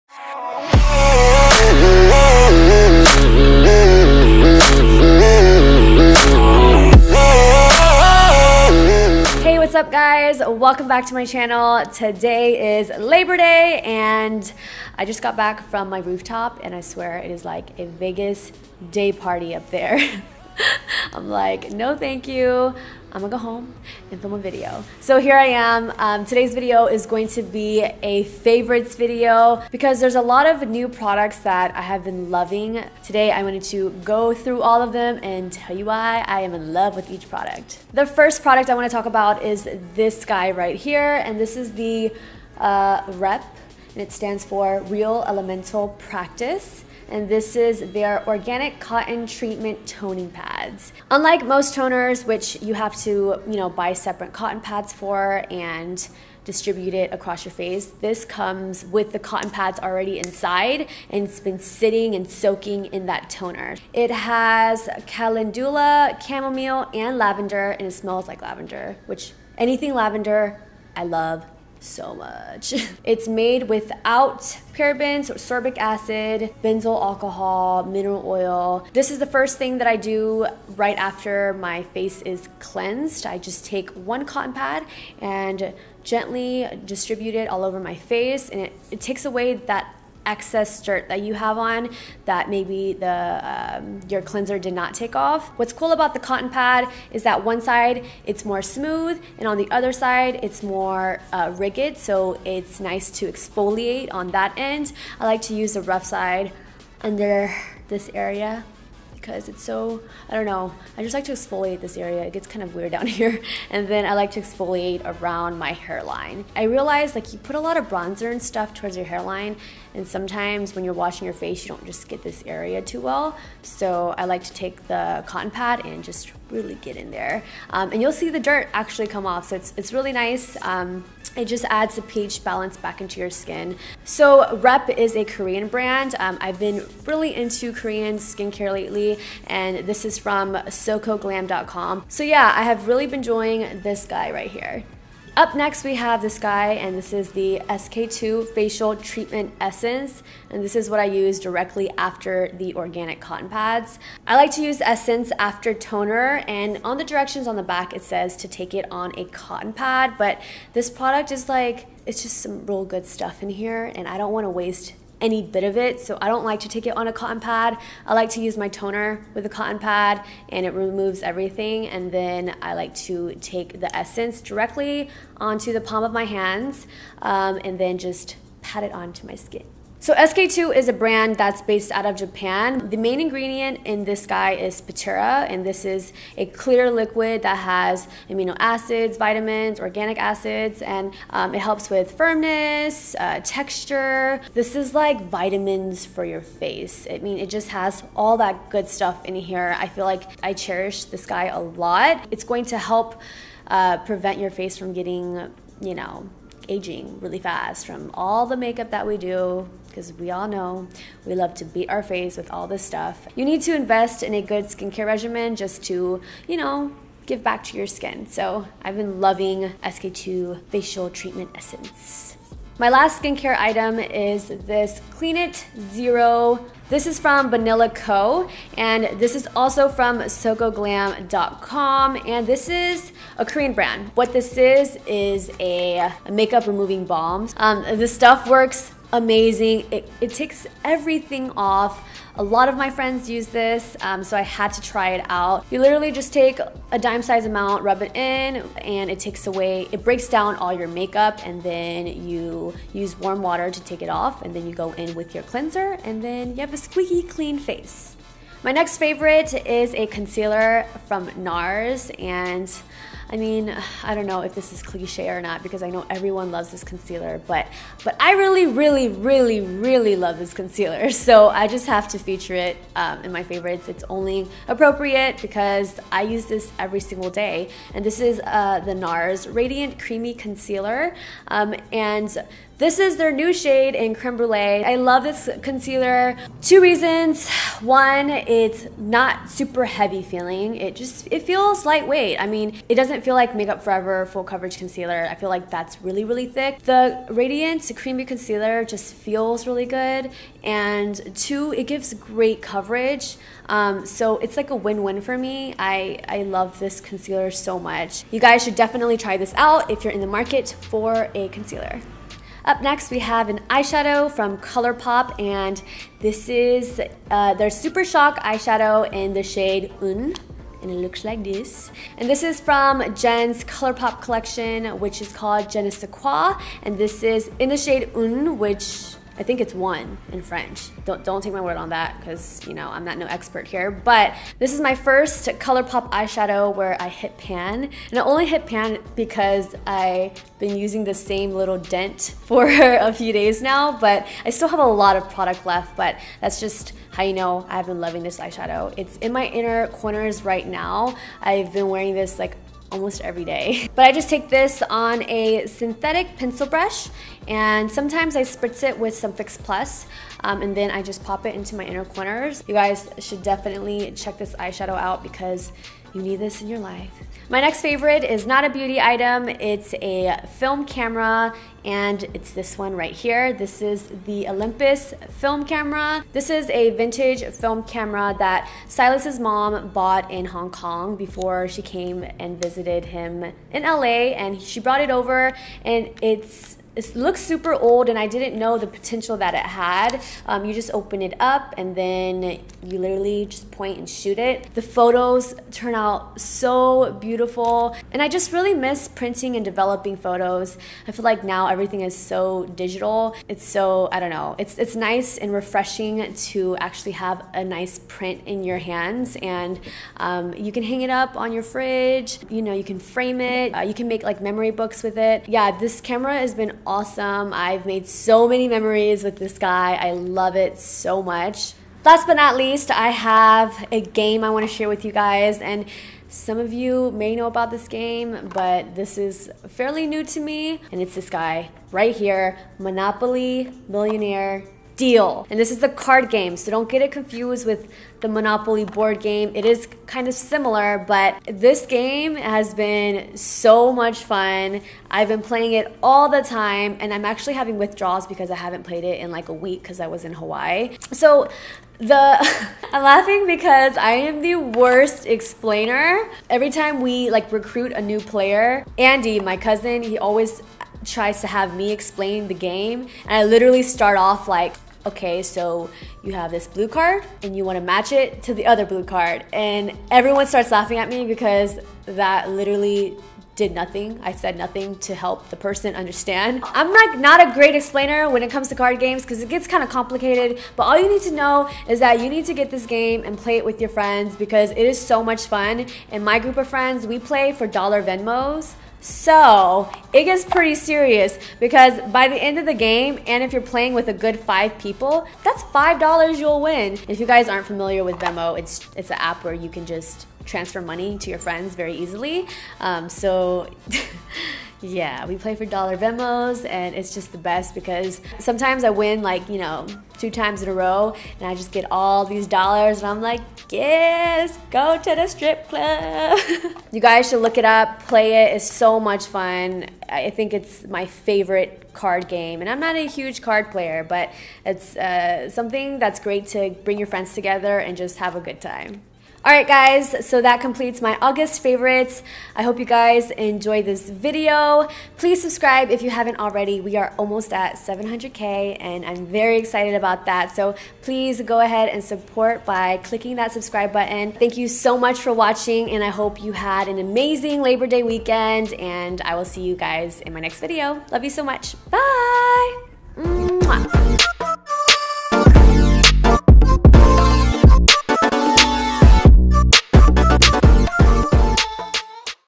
August Favorites
• PRODUCTS MENTIONED •

⇢ SK-II Facial Treatment Essence

⇢ Nars Radiant Concealer

⇢ Olympus Film Camera

⇢ Monopoly Deal